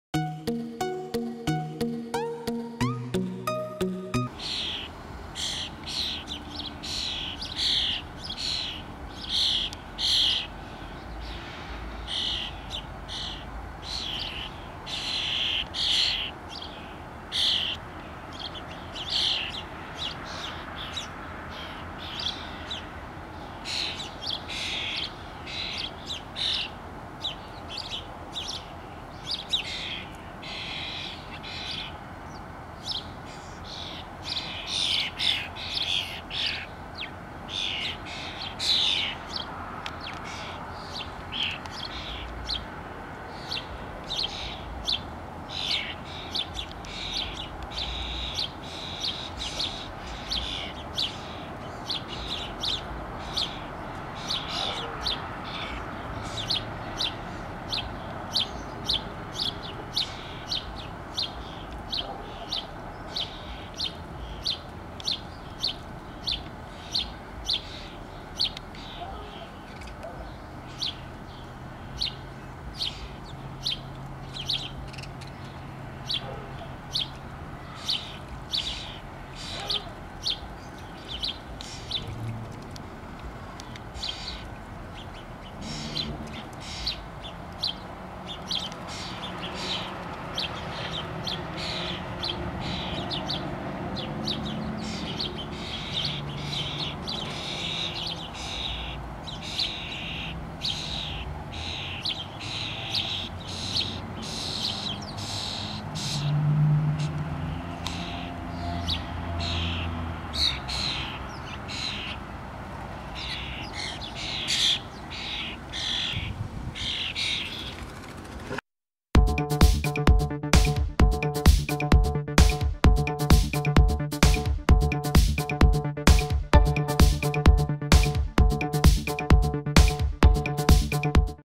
IECV NV #387 - 👀 Starlings Foraging Around The Yard And One Taking A Bath🐤 6-7-2017
Here we have some lovely Starlings out foraging around the backyard and one goes and takes a bath enjoy my Friends...If You're New To My Channel Be Sure To "Subscribe" "like" "Comment" "Share" "👍👍👍 Up" Thanks! For Watching And Commenting Have A Wonderful Week!